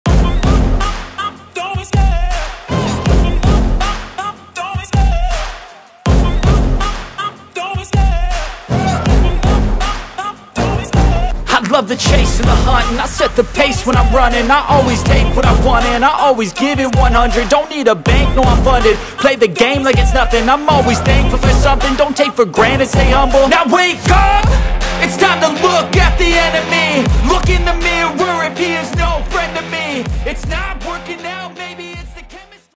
Vinyl Wrapping a Ford Bronco Sport Fender
Vinyl wrapping the fender of this new Ford Bronco Sport in 3M Matte Metallic Blue